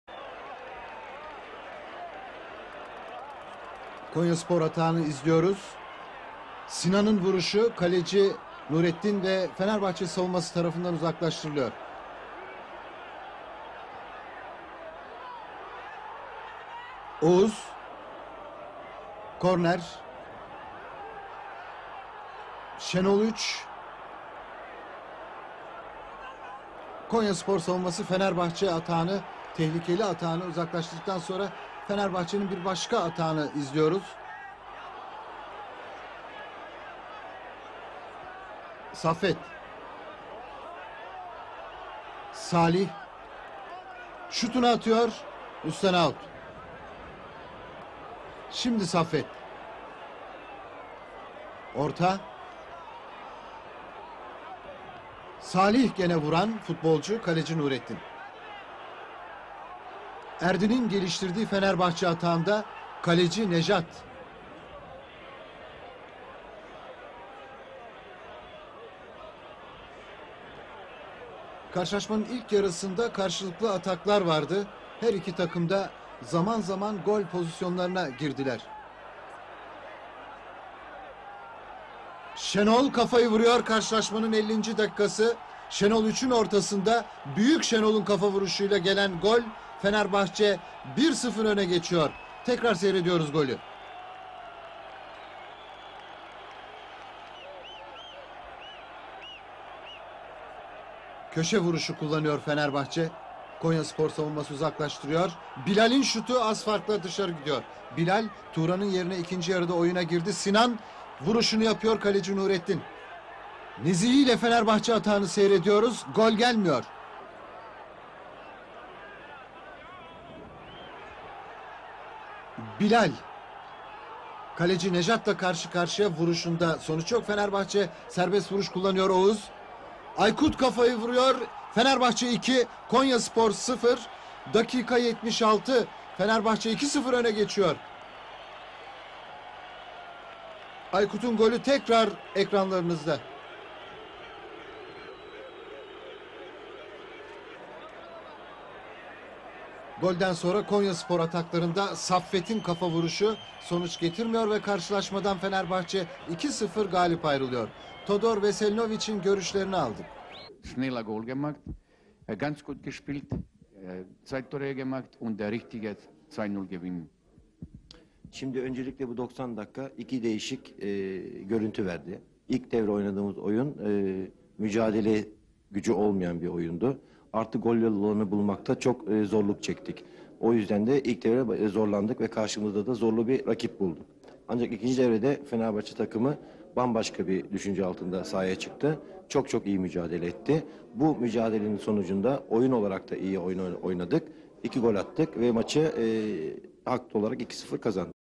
Fenerbahçe 2-0 Konyaspor [HD] 18.02.1990 - 1989-1990 1st League Matchday 20 + Post-Match Comments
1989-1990 Sezonu 1. Lig 20. Hafta Maçı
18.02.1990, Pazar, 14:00
Fenerbahçe
İstanbul, Türkiye